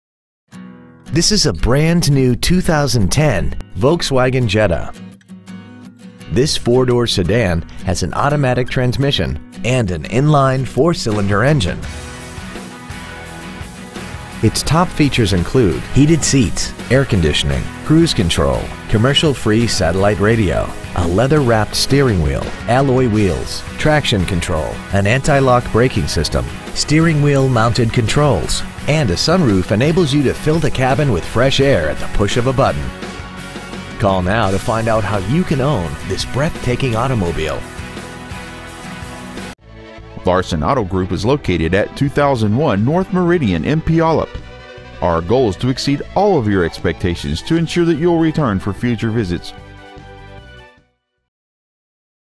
2010 Volkswagen Jetta Puyallup WA
Year: 2010
 Make: Volkswagen
 Model: Jetta
 Engine: 2.0L I4
 Trans.: Automatic
 Exterior: White
 Miles: 25
 Interior: Black
 Stock: J15880

 2010 Volkswagen Jetta Puyallup WA